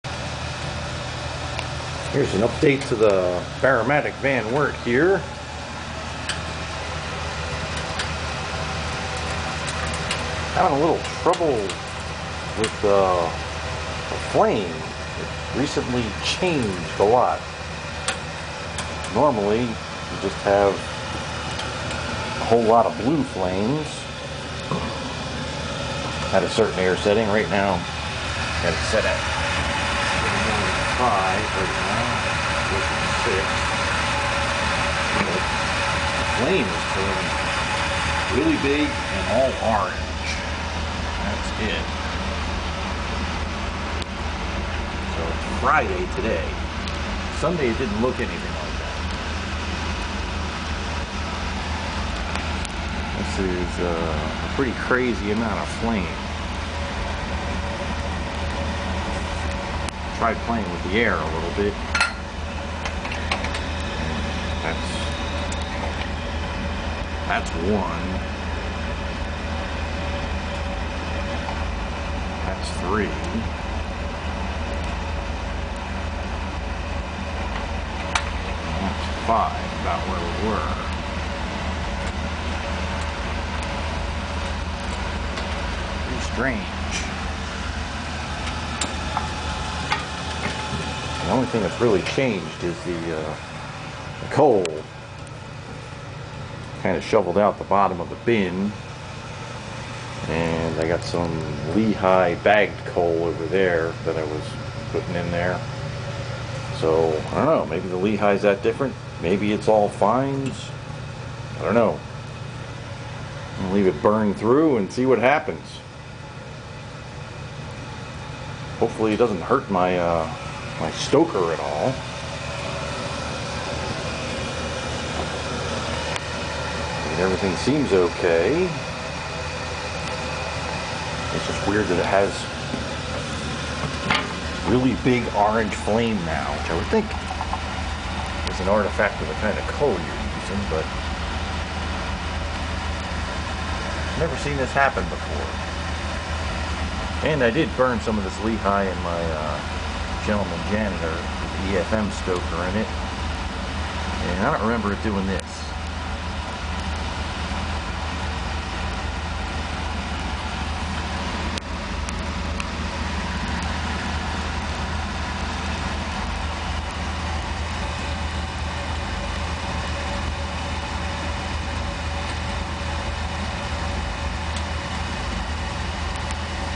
BairMatic / Van Wert Coal Stoker Boiler Flame Problems
Well, I don't know what the heck is going on here but my fire has suddenly changed, a lot. The flames are very big and very orange, they were smaller and blue.

The only things that have changed in the past week are:

1) Changed the aquastat from, LO 140, HI 160 to LO 120, HI 140. That should have nothing to do with the flame.

2) Shoveled out the bottom of the bin into the barrel. Then finished filling the barrel with bagged Lehigh. Lots of fines in both.

Could it be just the fines doing this?

Is the Lehigh that much different then the Blaschak I was burning?

I hope something hasn't gone wrong with the Van Wert. I played with the air setting to see if it helped, but it didn't.

-Don